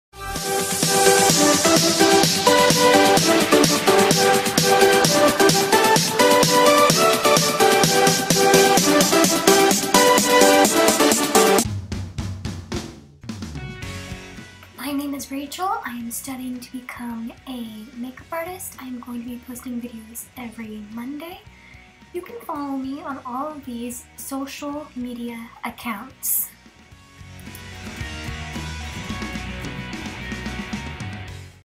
Welcome to my Channel trailer
here you fill fine me doing all sorts of fun things. you fill find me doing makeup and other cool things. I am going to be posting videos every Monday follow me on instagram so that you can get a shout out at the end of every video and you might see behind the sense of how I film and Any bloopers on My instagram